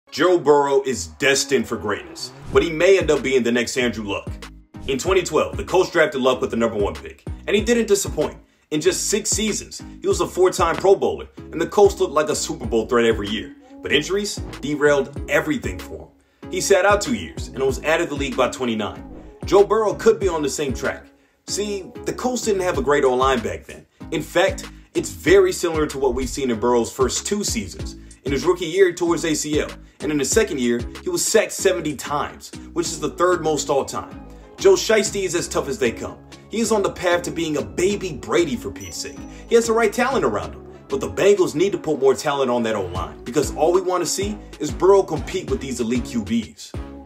Burrow Should Be Desperate For THIS
Joe Burrow is well on his way to becoming the next all-time great QB. He can be like aa Tom Brady or Peyton Manning, but he could end up becoming the next Andrew Luck. The Cincinnati Bengals better get the man some protection fast.